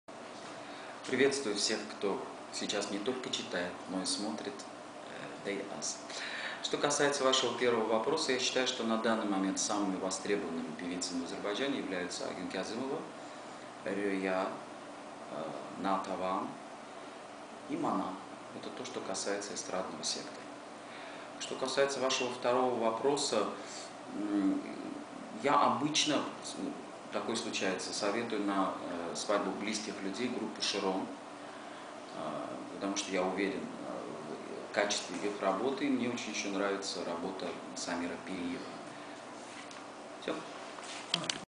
Фаиг Агаев в гостях у Day.Az Radio - Часть 2
Интервью Day.Az Radio с народным артистом Фаигом Агаевым.